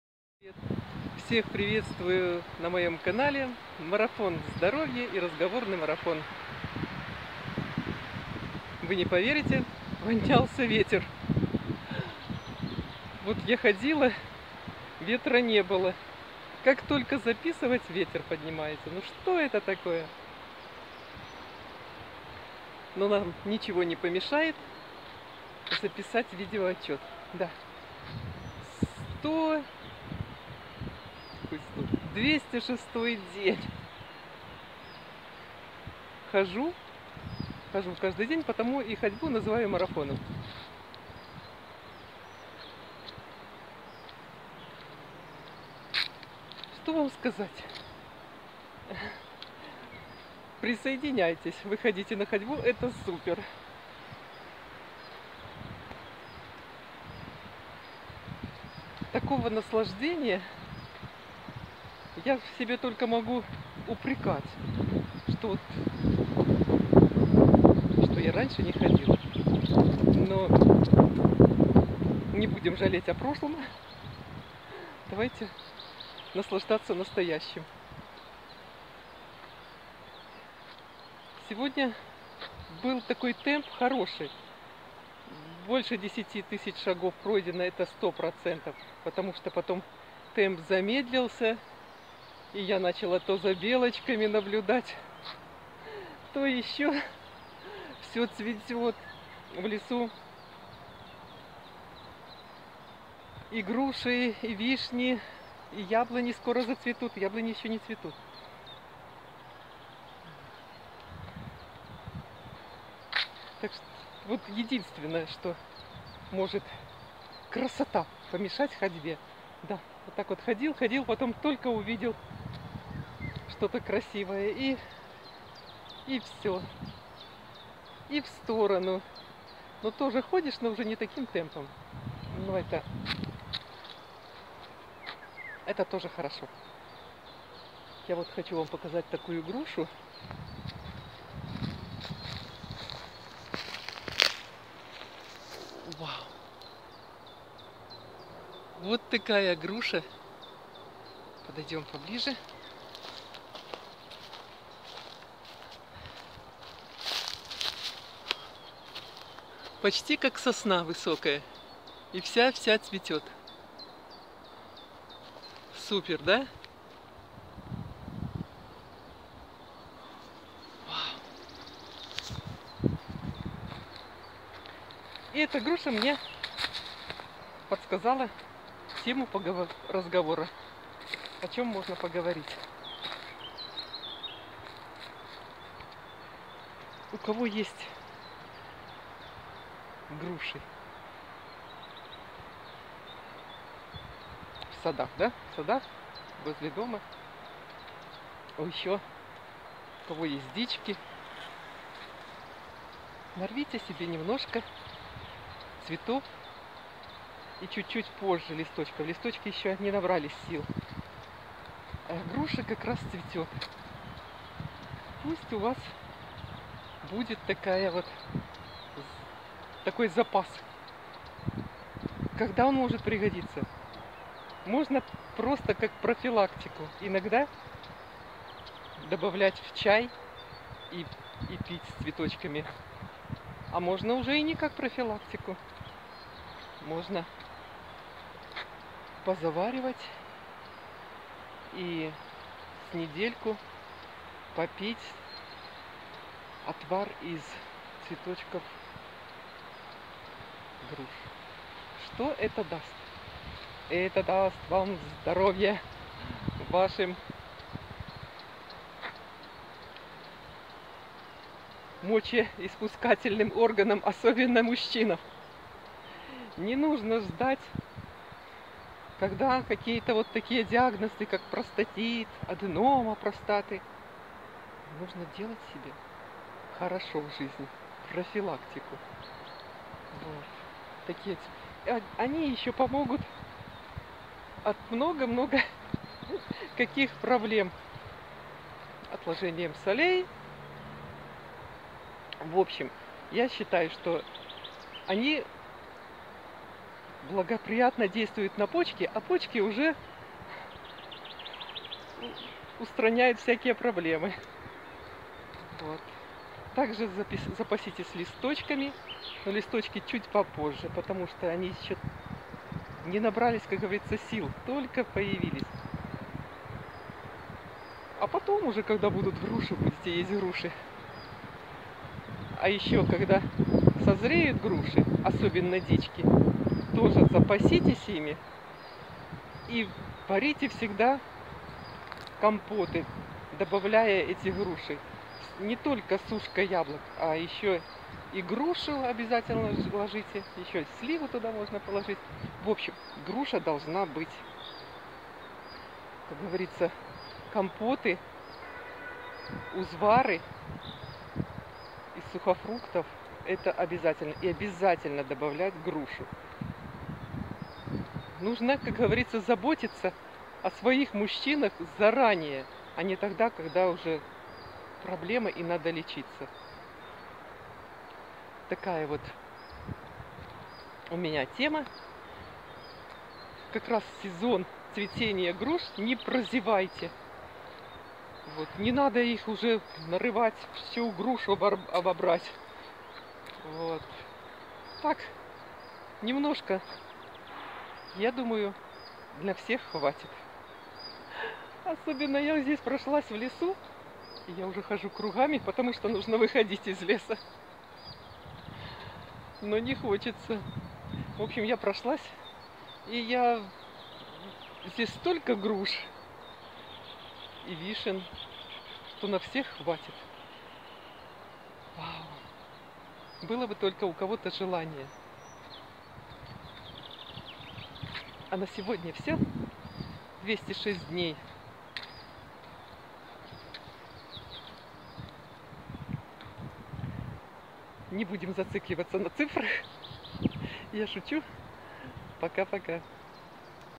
Цветы груши для профилактики! "Марафон здоровья и разговорный марафон" день 206
Привет! Всех приветствую на моем канале Марафон здоровья и разговорный марафон! Разговорный марафон: Сейчас цветет груша, успейте запастись ее цветами, а позже нарвать и листочков. Цветочки помогут избавится от многих болезней. Особенно полезно для мочеиспускательной системы мужчин. Не нужно дожидаться страшных диагнозов. Употребляйте цветы, листья и плоды груш для профилактики!